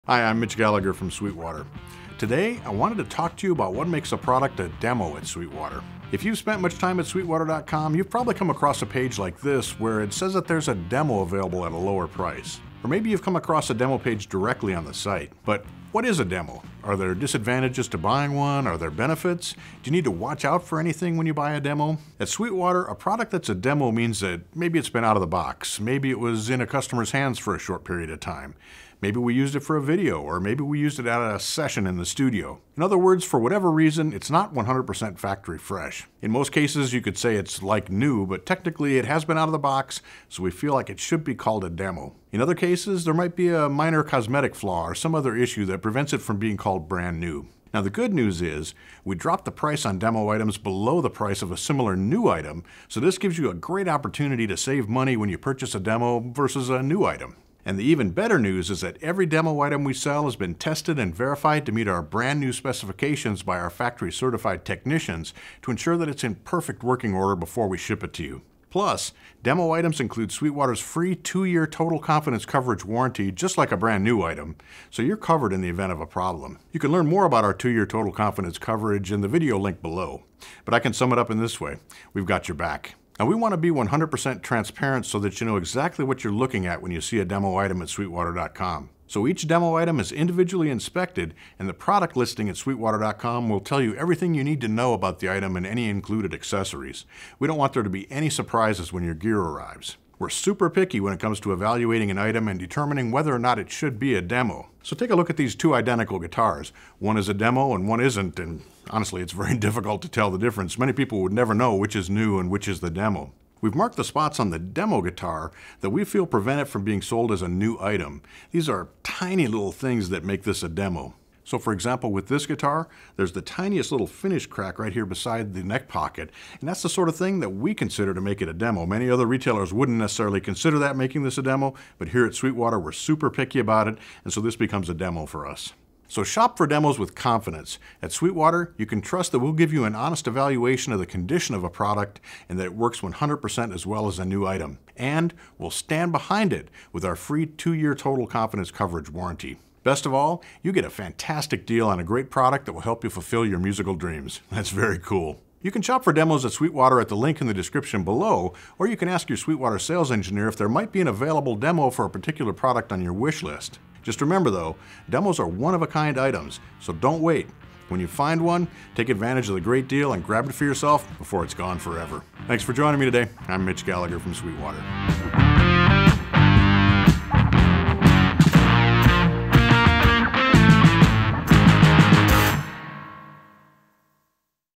What Makes a Sweetwater Demo Item?
If you’ve ever felt skeptical when a musical instrument retailer touts attractive pricing by calling something a “demo,” a “B-stock item,” or something similar, then you’d certainly be forgiven. Far too frequently, musicians end up overpaying for used instruments, receiving items with dings, damages, or other detritus that don’t match what they’ve been told to expect, whether the discrepancy is in the price or the quality. Fortunately, you can rest assured that Sweetwater catalogs usage and wear on every item with exacting precision. For example, guitars can be viewed on the basis of individual serial numbers, receiving special designations for photos to illustrate any possible wear. Everything from amps, speakers, and commercial audio to synthesizers, recording consoles, band and orchestra instruments, and virtually anything else we carry can be sold as a demo item. But what really makes a demo item here at Sweetwater? Join Mitch Gallagher to learn more about the care and consideration we practice in guaranteeing the best possible prices for all non-new items!

After you watch, check out Sweetwater.com today for all your music instrument and pro audio needs!